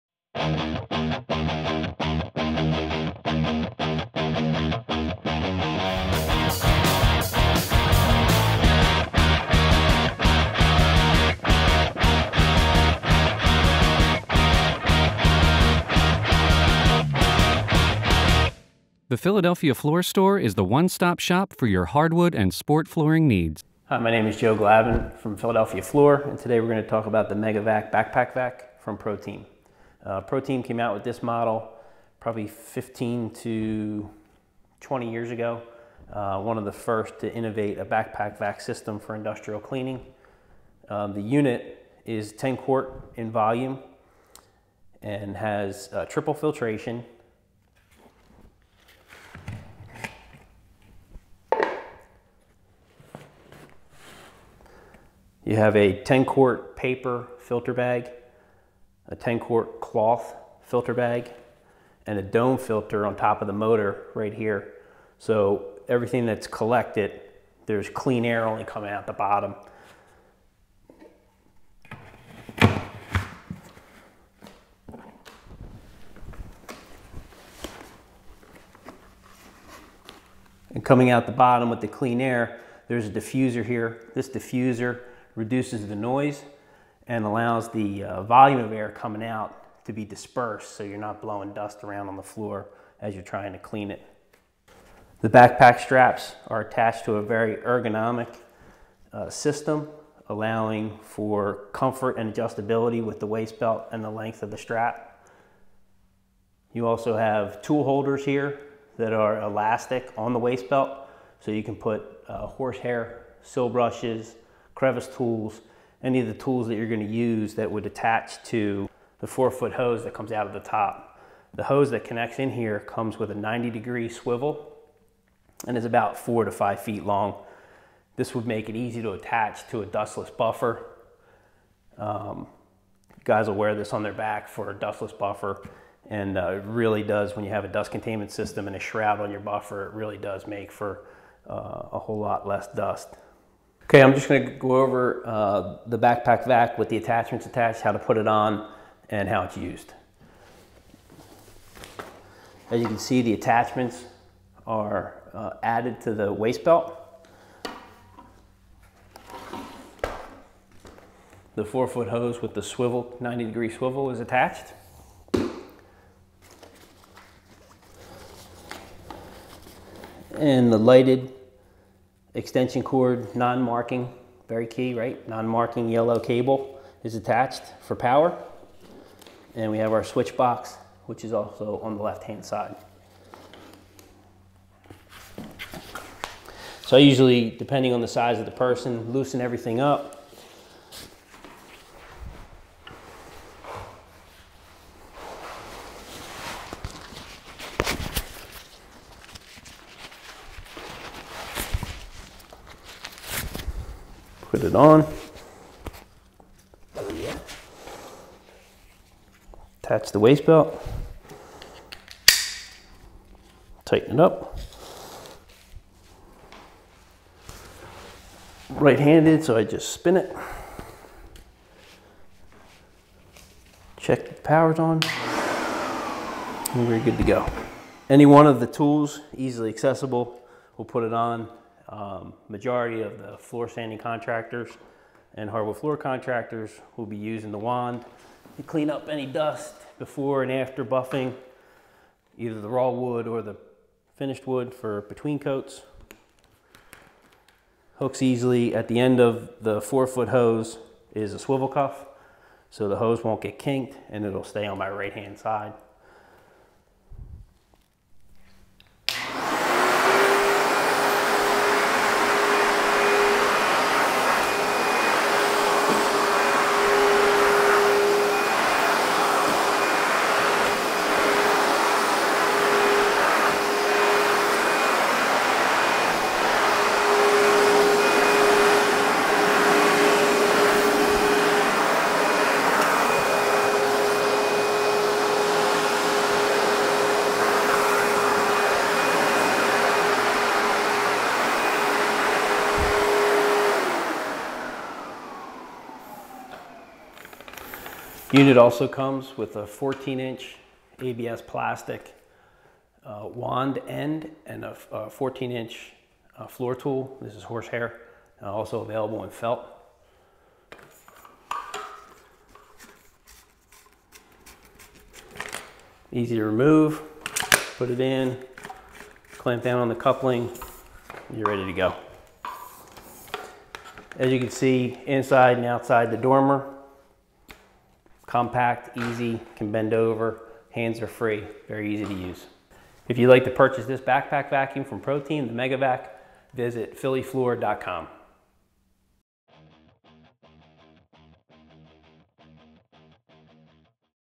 Pro Team MegaVac Industrial Vacuum | City Floor Supply
This video highlights the features and benefits of ProTeam's MegaVac backpack vacuum.

See how the versatile design allows for quick conversion into a high powered blower while the huge filter capacity contains large amounts of debris.

Pro Team was one of the first companies to create a backpack-style vacuum for industrial cleaning purposes, and this model is still one of the most innovative on the market.

This is a great piece of equipment for hardwood floor contractors to use because it connects easily to dustless buffers, and its powerful motor allows for efficient cleanup before and after the sanding process.

The MegaVac has a containment volume of 10 quarts, which allows you to keep going longer. This vacuum features a HVAC triple filtration system which allows only clean air to come out of the vacuum.

The backpack strap system is very adjustable and ergonomic. The waistband features tool holders for easy access to accessories. The power switch is also connected to the waistband for added ease of use.

The vacuum's 4-foot hose comes with a 90 degree swivel, making it easy to attach to a dustless buffer.

Attach the wand tool, which hooks easily onto the hose, for added reach when cleaning up before and after the flooring installation process.

Browse our full inventory, shop 24/7, and earn rewards just for buying the supplies you need